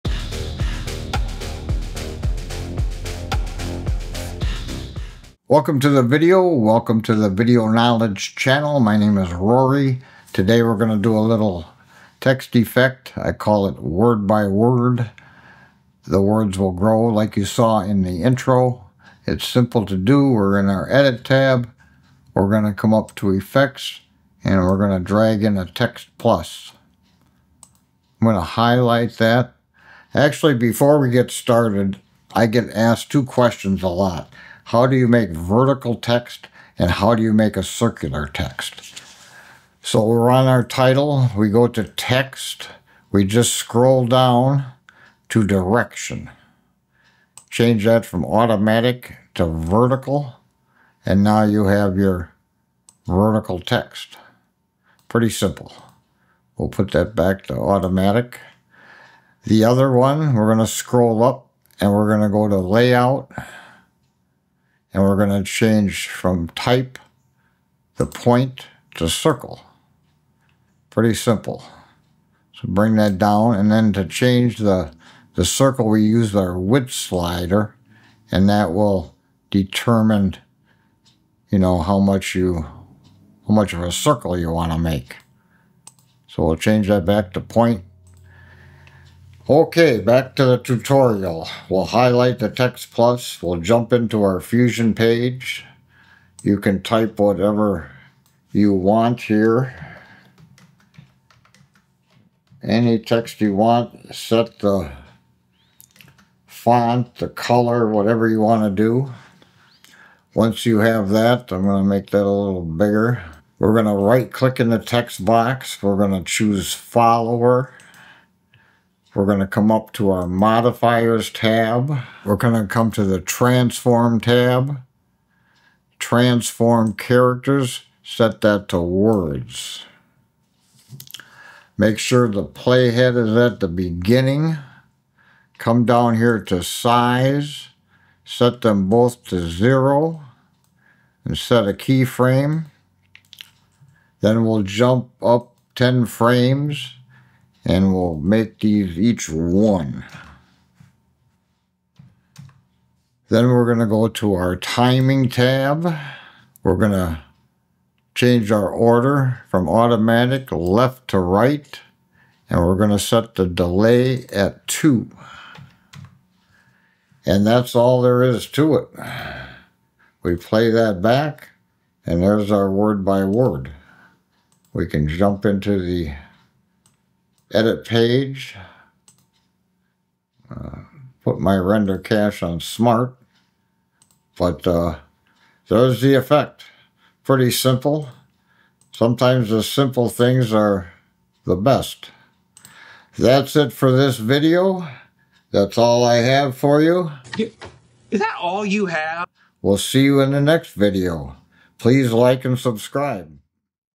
How to Make TEXT GROW & APPEAR Word by Word in DaVinci Resolve (Beginner Friendly)
Learn how to create a stunning animated text effect in DaVinci Resolve! In this step-by-step tutorial, I'll show you how to make words appear one at a time with a smooth scaling animation - starting small and growing to full size for maximum visual impact.
🎯 What You'll Learn:

How to animate text in DaVinci Resolve
Creating sequential word animations
Scaling effects for dynamic text reveals
Timing and keyframe techniques
Professional text animation workflow

This simple yet effective text effect is perfect for:
✅ YouTube intros and outros
✅ Social media content
✅ Promotional videos
✅ Title sequences
✅ Presentations and slideshows
Perfect for beginners! No advanced skills required - just follow along with this easy DaVinci Resolve tutorial.

Software Used: DaVinci Resolve (Free Version Compatible)